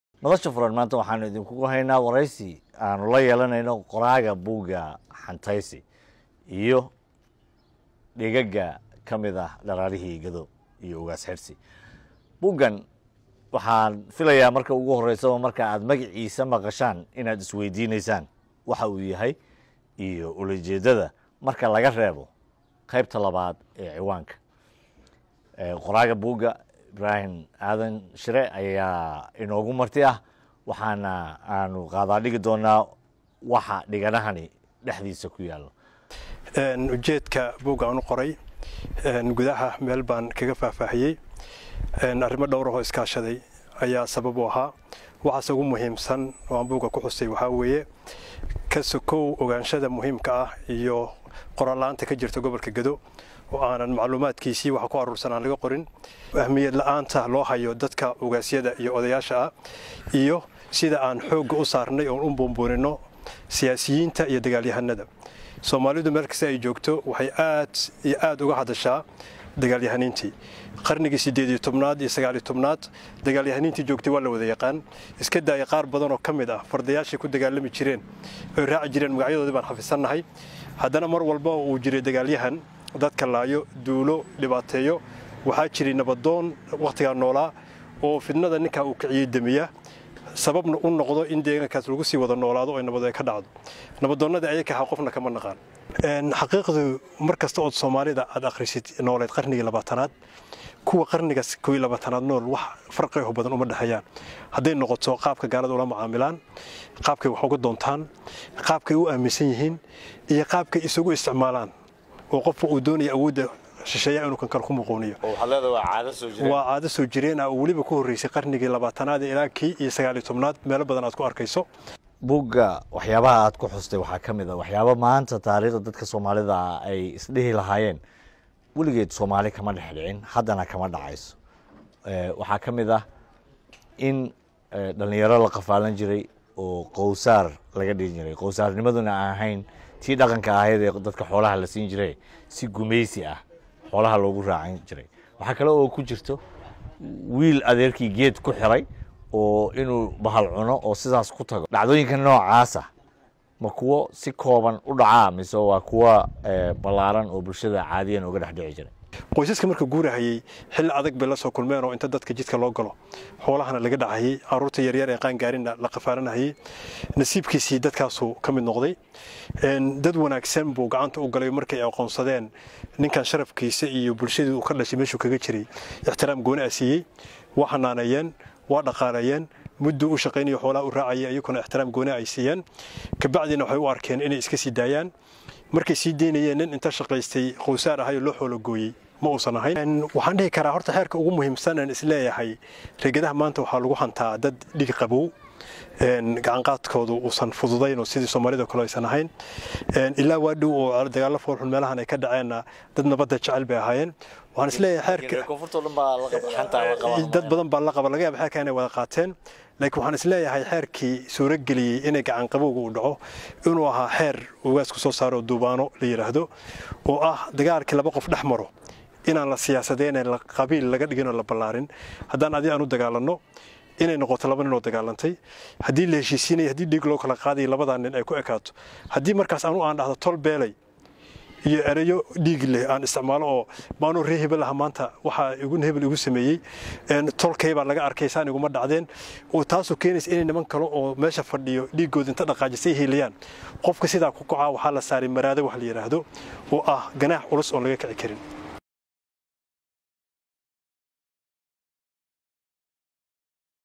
Madasha Furan iyo Buugii Xantaysi
Dheygag ka mid ah dharaarihii gedo iyo Ugaas Xirsi, waa buug uu qoray qoraa Ibraahim Aadan waxaana wareysanaya wariye Jamac Buule.